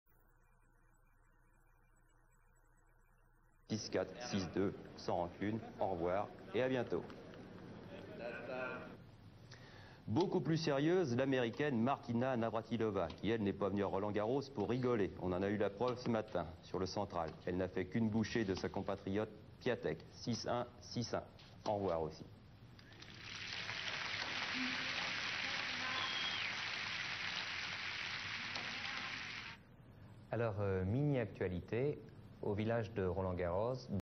Mary Lou Piatek Daniels vs Martina Navratilova, 1983 French Open
Martina Navratilova defeated Mary Lou Piatek 6-1 6-1 in the first round of the 1983 French Open